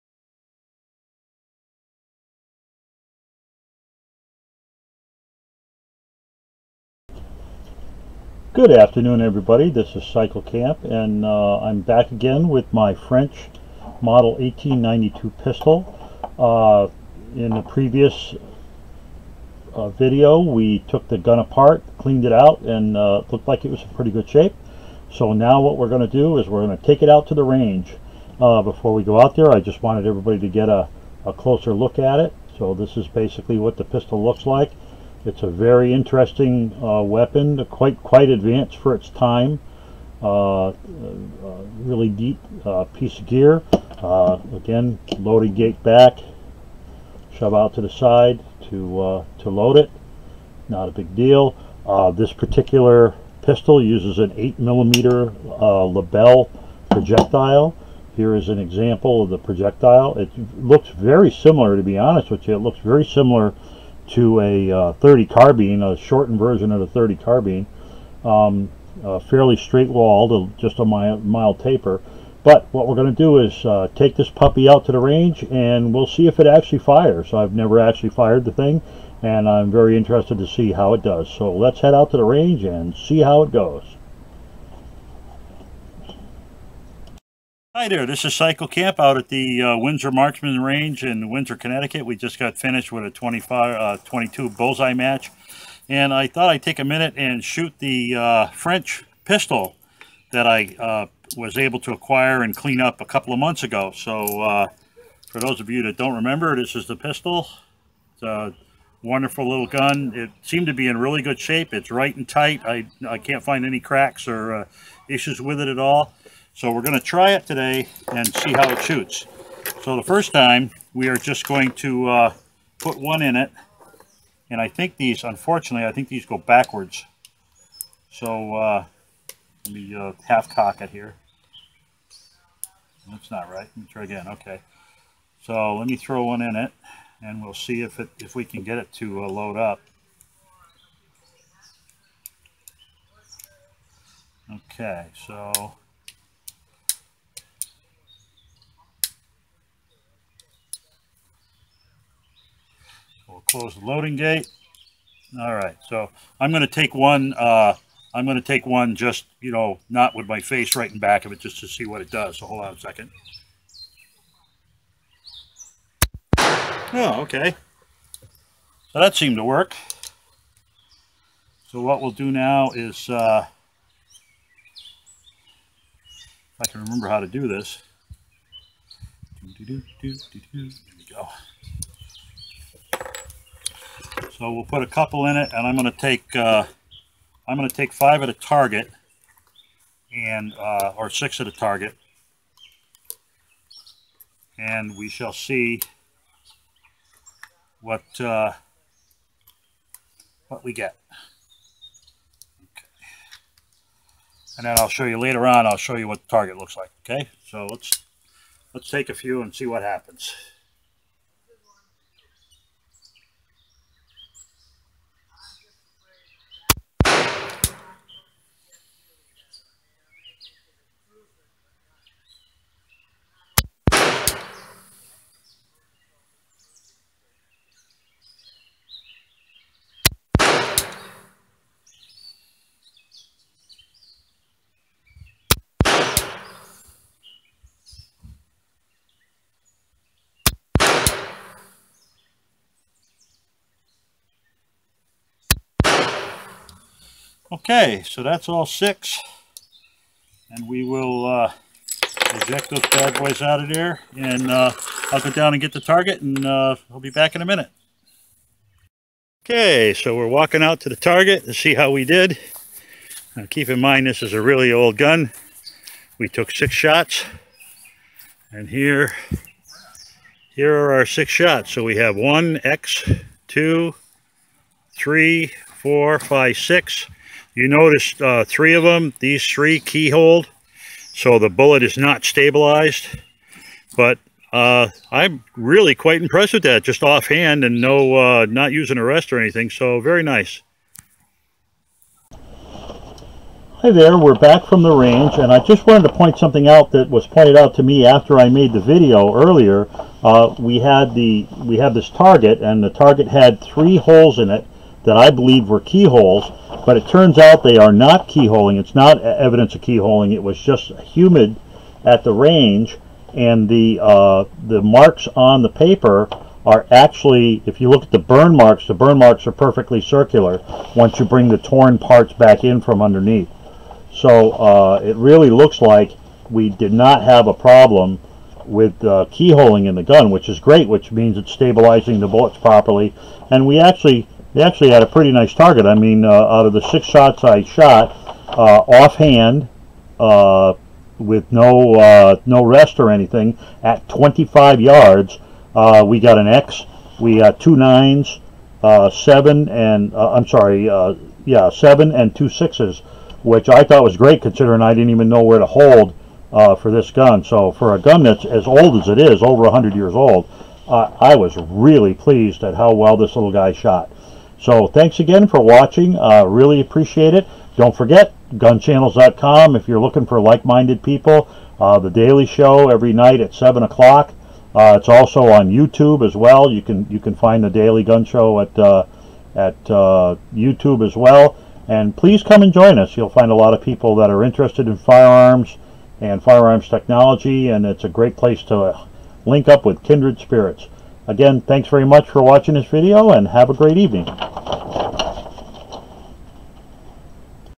French MLE 1892 at the range
Shooting the French MLE 1892 pistol made in 1912 at the range, after dis-assembly and cleanup.